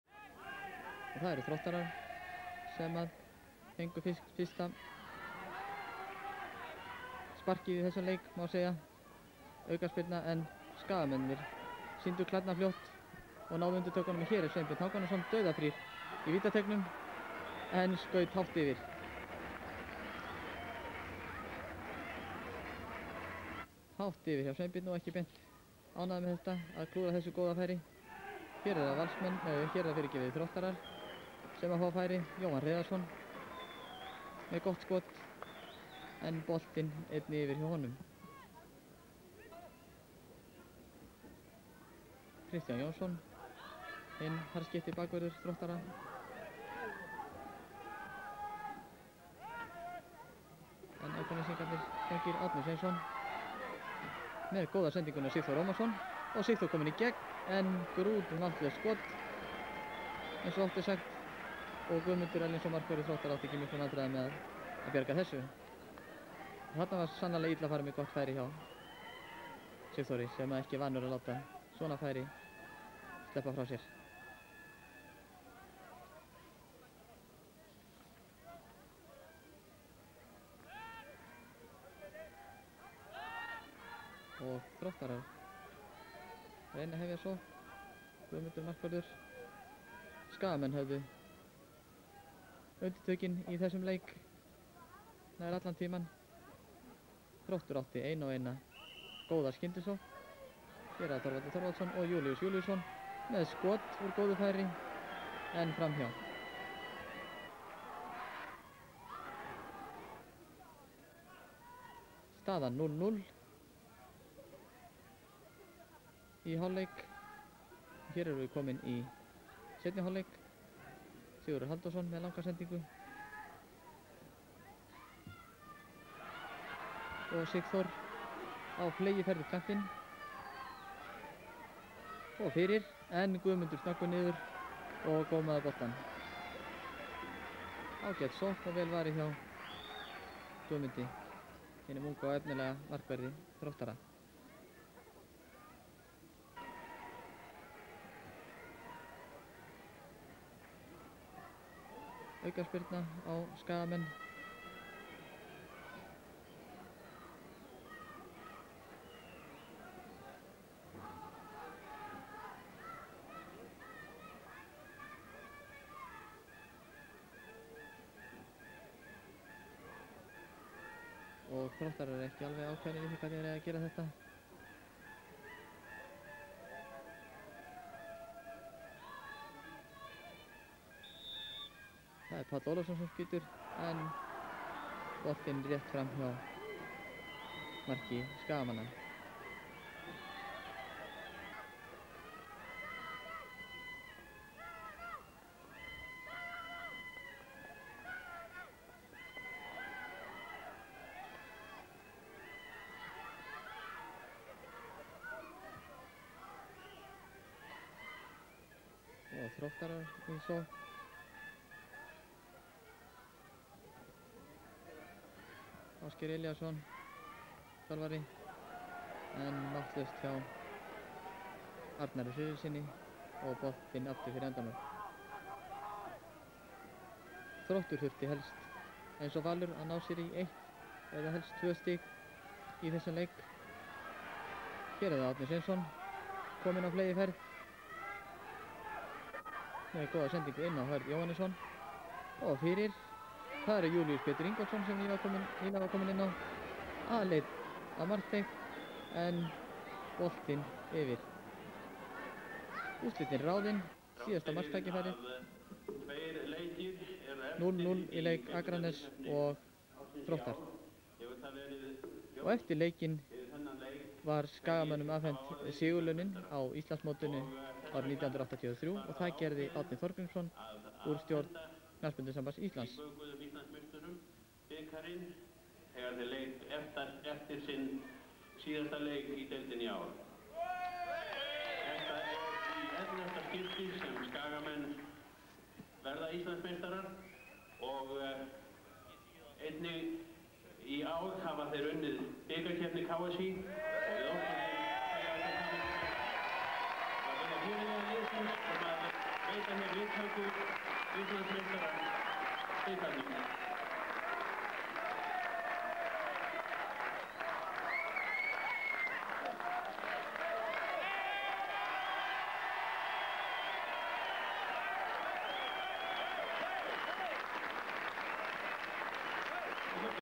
Þróttur R. - ÍA 1. deild karla 1983
RÚV Íþróttir / Umsjón: Ingólfur Hannesson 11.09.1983 / 18. umferð / Laugardalsvöllur / Þróttur R. - ÍA 0:0